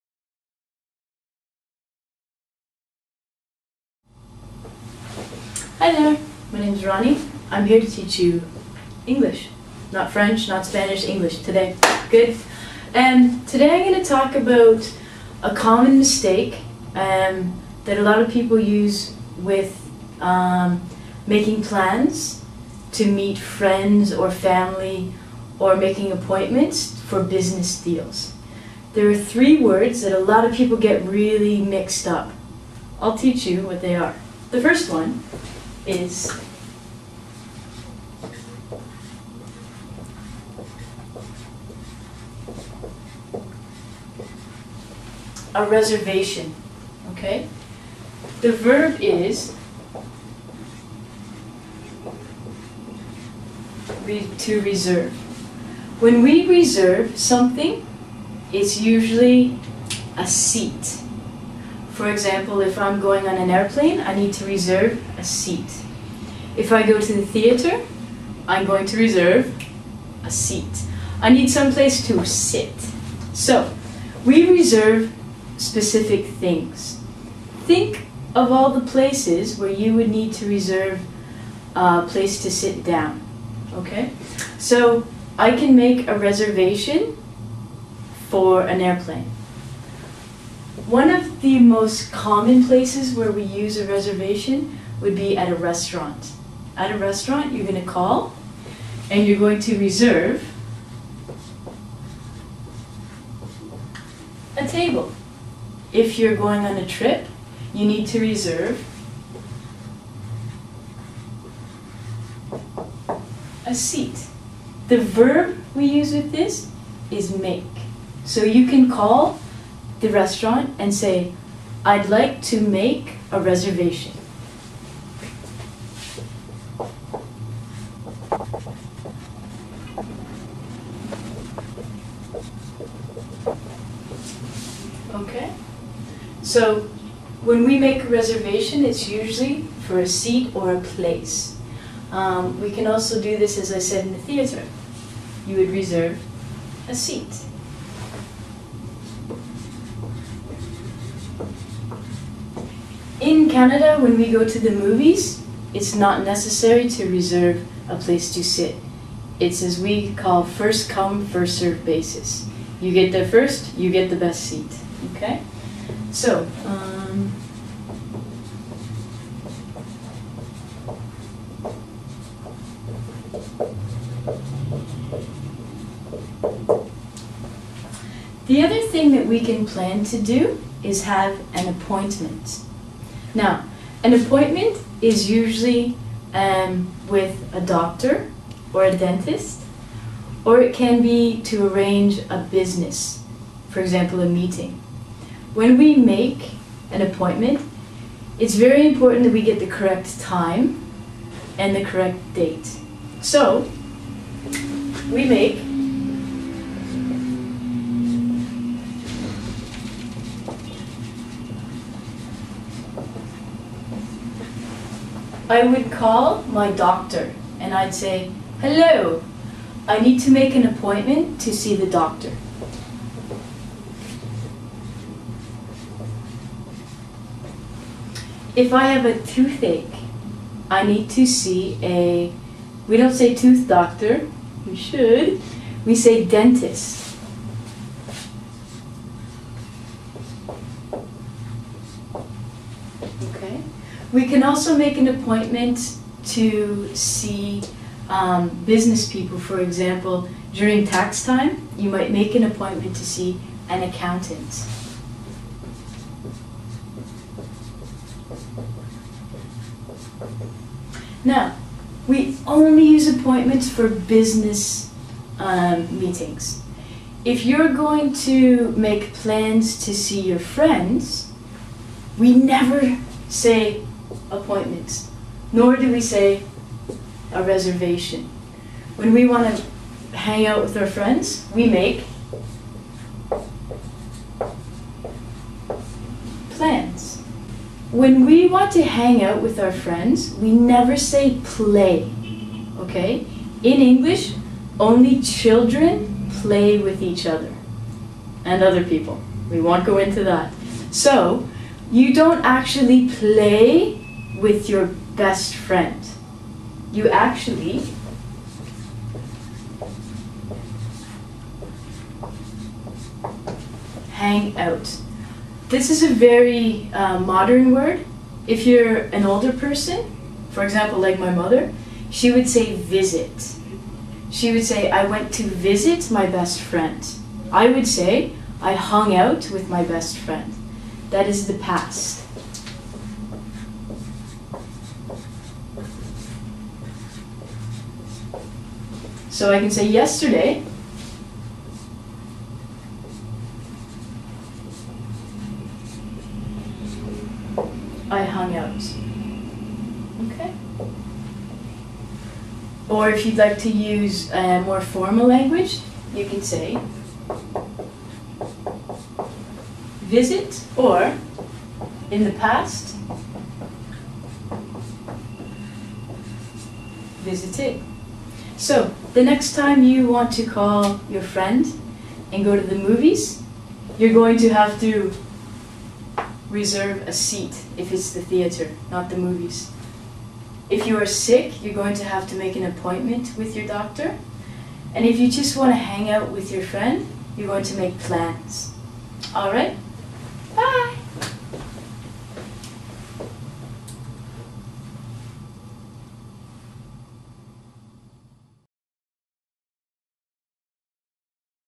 English Vocabulary - Appointments & Reservations
How do you make an appointment? This lesson is going to sort out any confusion you have about appointments or reservations.